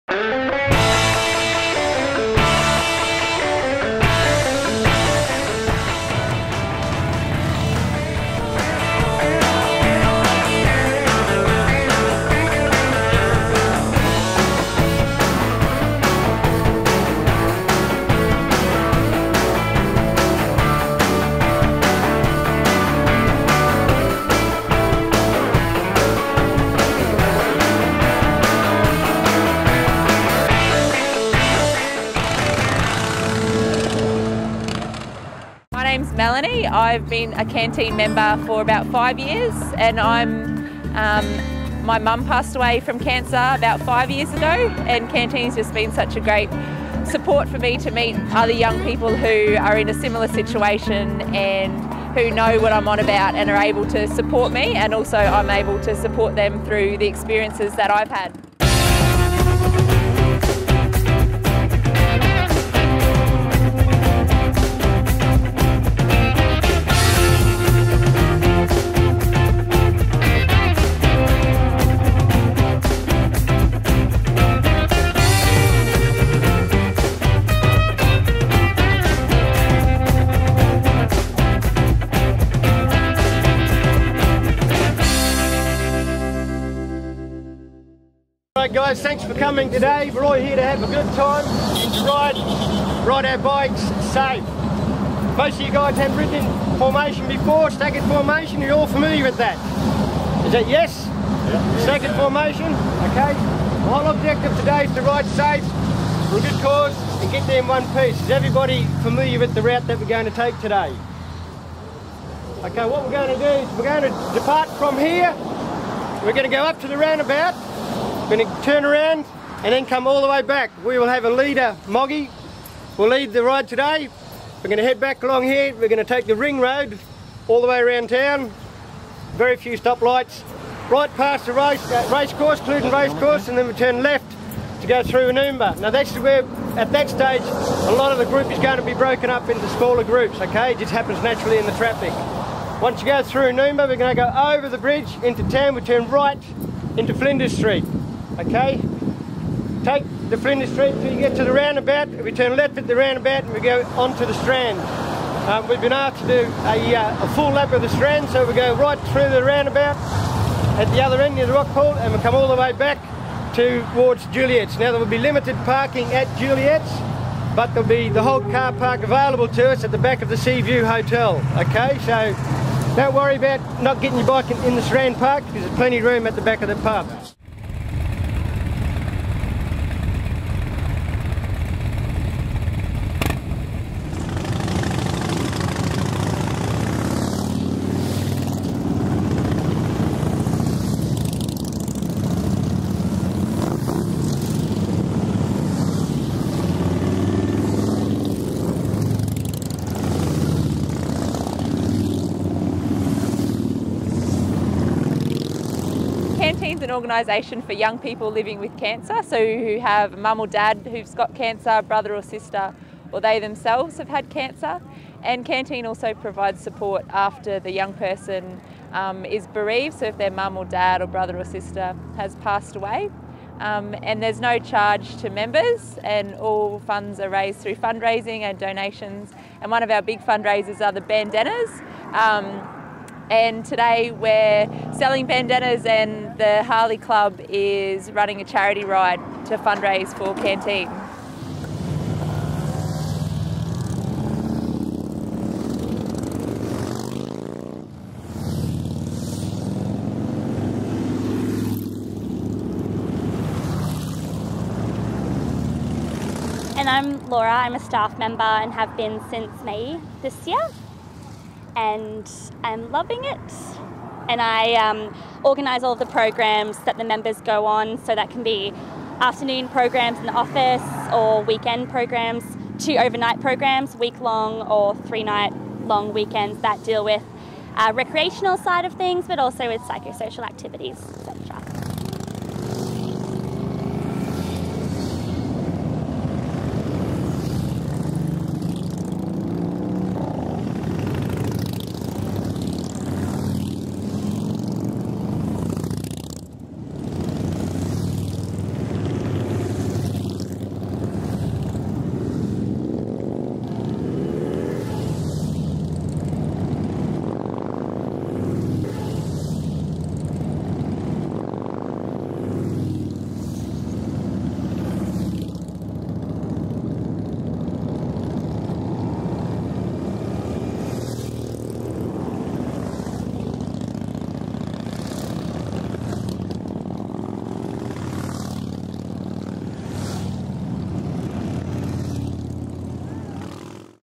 Suncity Townsville's Bandana Ride Part-1
SunCity Harley-Davidson, Townsville, ran their first annual CanTeen Charity Ride, which raised much-needed funds to support young people living with Cancer. Here in Part-1 of the "2011 Bandana Ride" see some of the 150 bikes that joined in the ride leaving the new Sun City Harley-Davidson dealership.